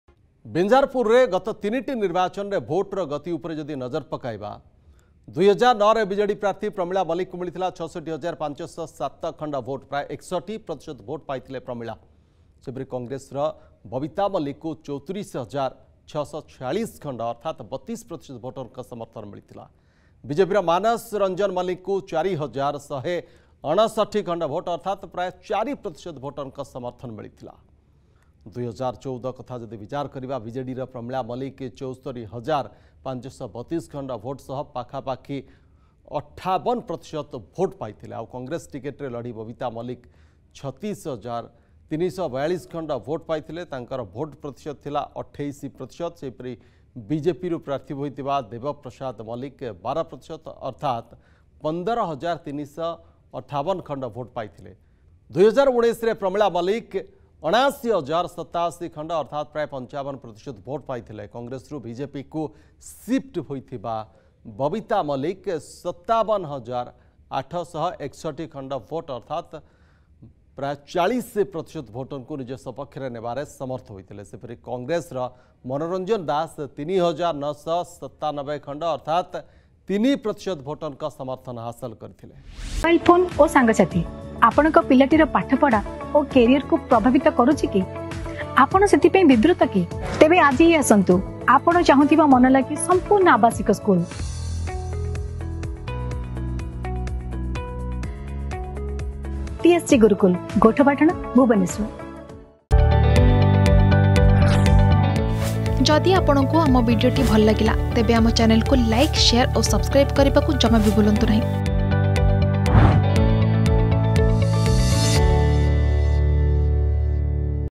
Janata Mood || ବିଞ୍ଝାରପୁର ନିର୍ବାଚନ ମଣ୍ଡଳୀରେ ଭୋଟର ଗତି ଆଜି ଯାଏଁ କ’ଣ ରହିଛି
ବିଞ୍ଝାରପୁର ନିର୍ବାଚନ ମଣ୍ଡଳୀରେ ଭୋଟର ଗତି ଆଜି ଯାଏଁ କ’ଣ ରହିଛି ।